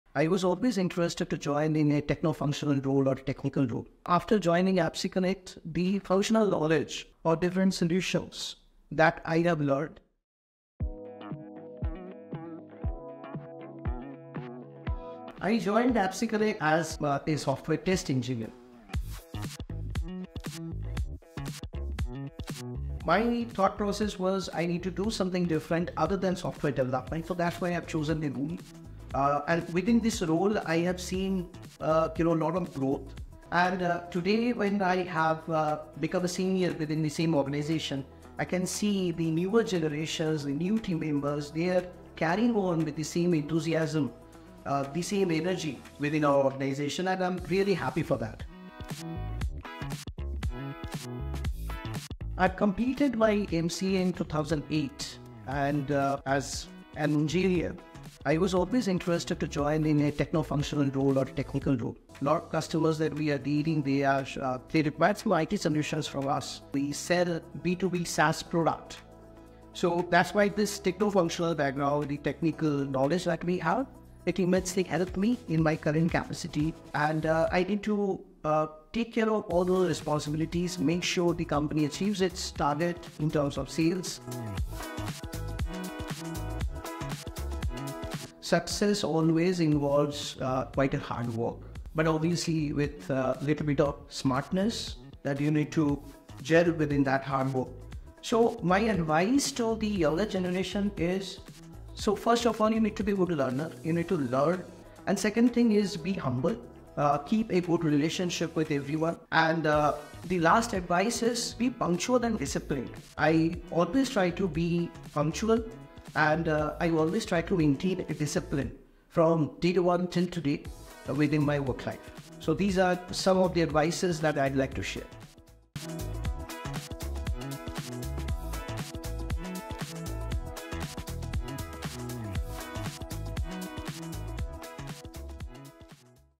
Journey at APPSeCONNECT | Subhajit Goswami | Software Test Engineer to VP of Business Development
Watch this inspiring video of Subhajit Goswami, the Vice President of Business Development at APPSeCONNECT, as he reflects on the challenges, triumphs, and invaluable lessons learned along the way.

Starting as a software test engineer, Subhajit's dedication, hard work, and strategic mindset propelled him through the ranks to his current leadership position.

Whether you're navigating your own career path or seeking inspiration for professional growth, Subhajit's story offers valuable insights and encouragement.